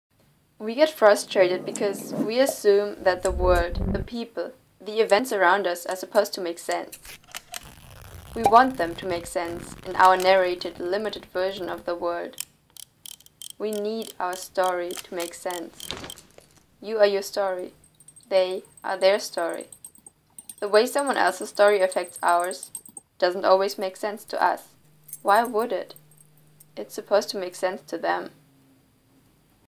You are your stories
The first video in.. 2 months? 3 months? I don't even know anymore. I started uni and found myself with too little time and not enough ability to decide what's important to me (then again, to be honest, I think at times it is perfectly fine to spend your life wih nothing but socialising and watching movies.. and homework... I didn't even watch any youtube in the past 3 months, something I never could have imagined a few years/months back, I mean, I love youtube!)
I feel kinda bad that this video is... not as amazing as I'd want it to be after such a long break but I figured I should start somewhere. I tried doing some animations which is something I'm only jsut learning - and loving. I could have uploaded my animation homework but... that would've been kinda pointless because I want to create cool(ish...) stuff and say something, I don't want to just show you bouncing balls and walk cycles... :)
Thank you for being awesome xoxo